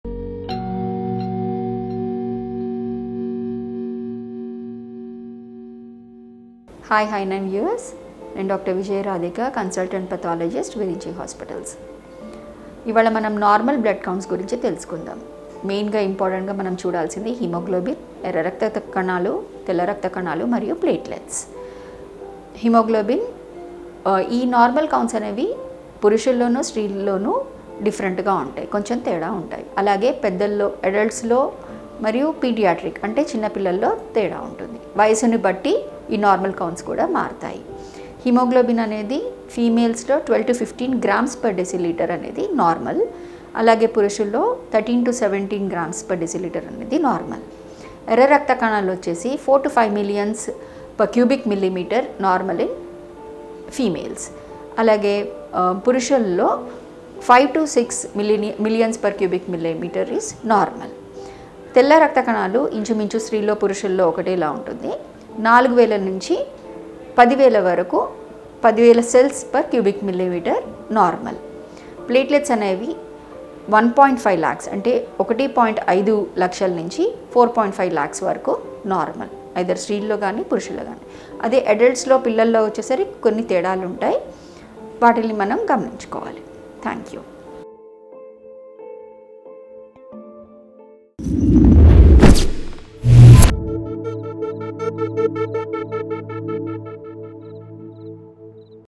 Hi9 | సాధారణంగా రక్త శాతము ఎంత ఉండాలి? | Dr R Vijaya Radhika | Consultant pathologist
Normal Blood Count in Telugu from Hi9 by Dr R Vijaya Radhika Consultant Pathologist and Lab Director
A complete blood count, or CBC, is an easy and very common test that screens for certain disorders that can affect your health. A CBC determines if there are any increases or decreases in your blood cell counts.The complete blood count (CBC) is one of the most commonly ordered blood tests.A complete blood count (CBC) is a test that measures the cells that make up your blood: red blood cells, white blood cells, and platelets.